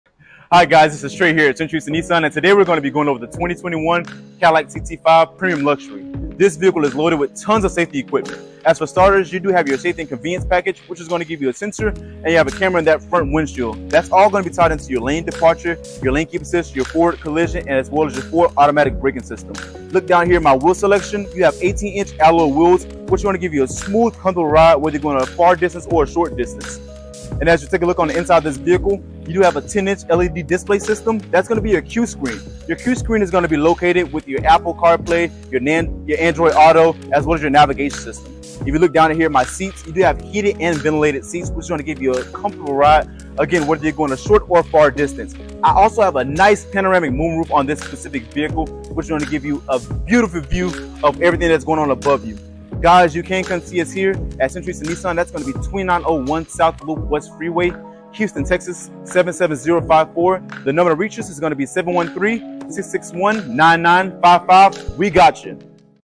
Cadillac Pre-Owned
Listen to Trey from CentralHoustonNissan present the gorgeous Cadillac CT5 Premium Luxury and all of it's fantastic features! 🤩

This ride is jam-packed with tons of safety features and luxurious details to make your drive as comfortable as possible.

👉 Buy it before it's gone!